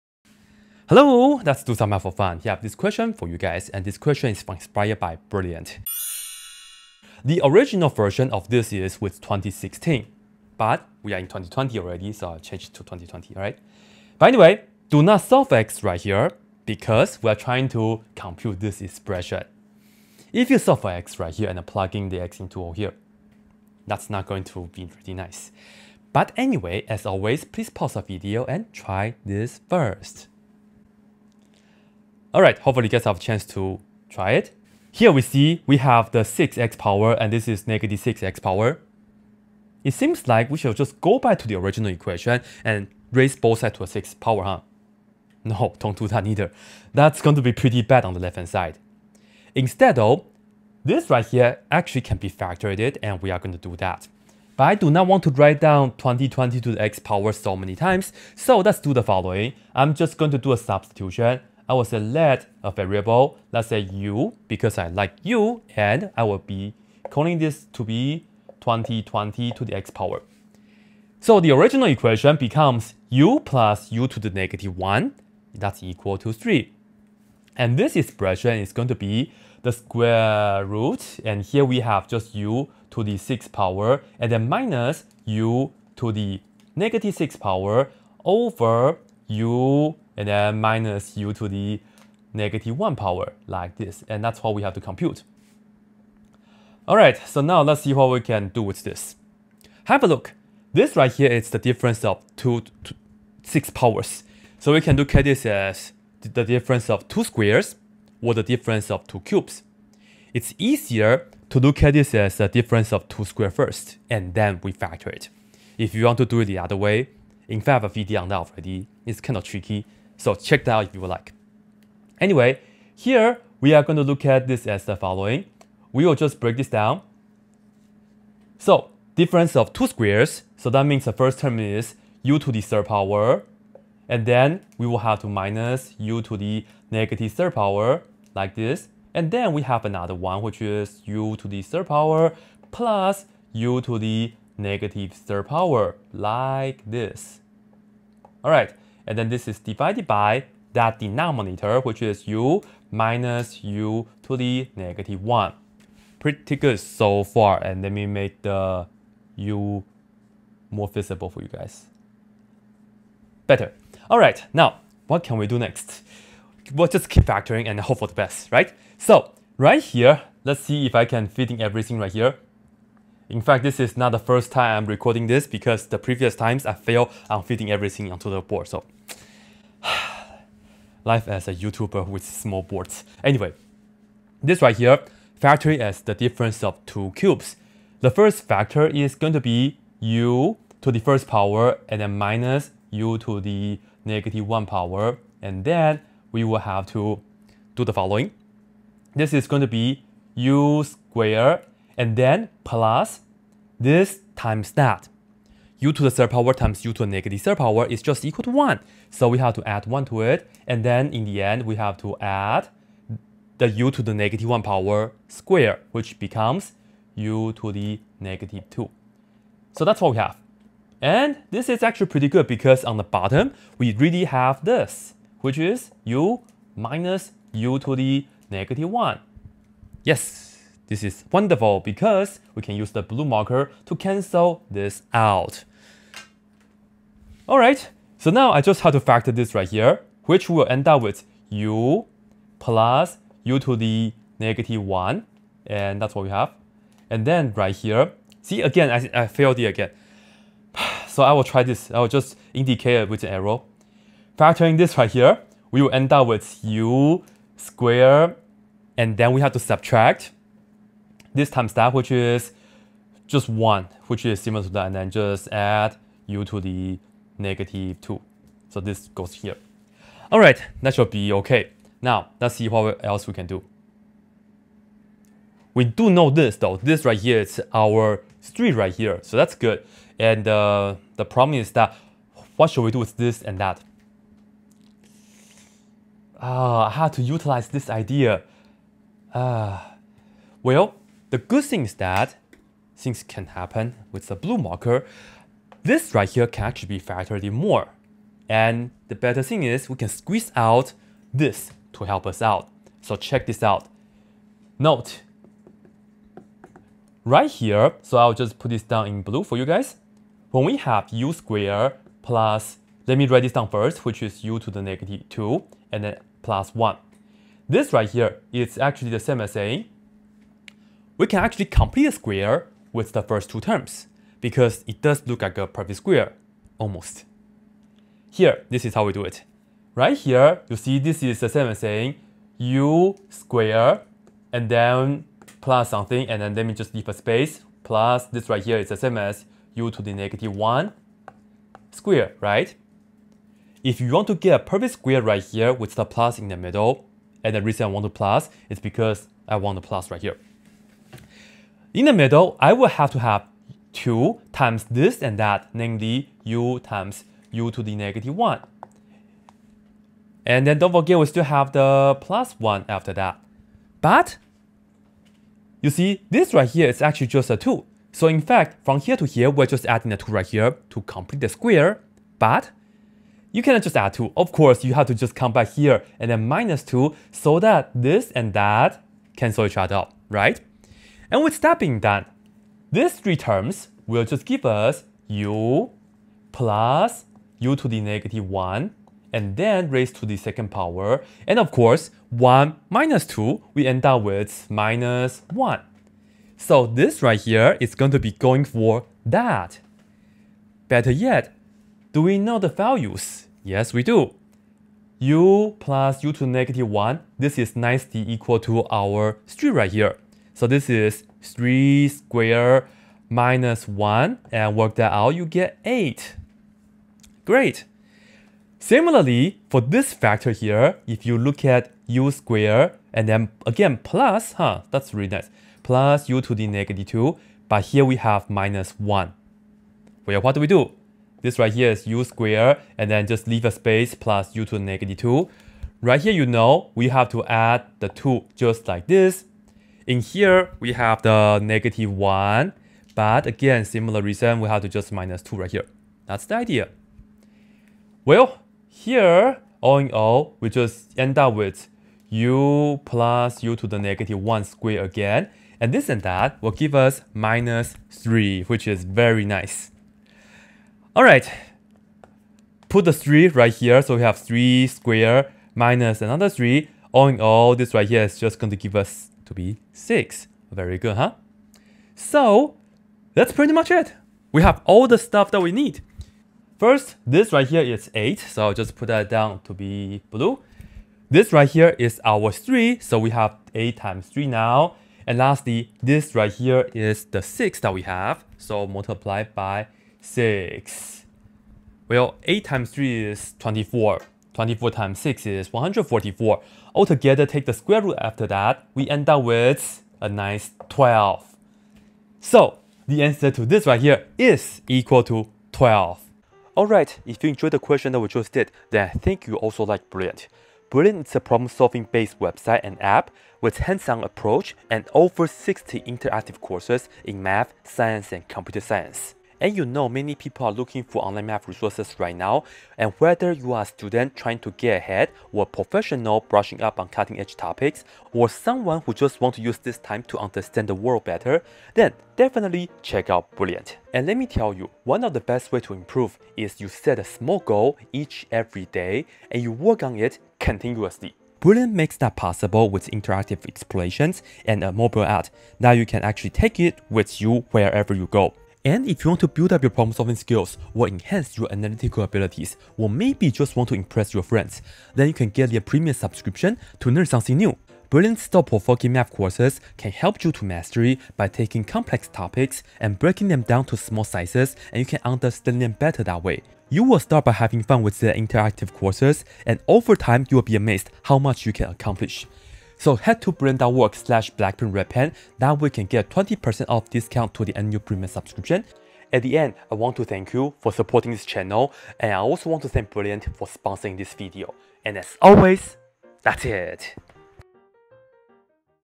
A Brilliant Algebra Problem
We are given an exponential equation 2020^x+2020^-x=3 but we just have to evaluate that BIG square root expression. It is NOT a good idea to solve for x, but rather we should think about how we can do some "brilliant" algebra manipulation.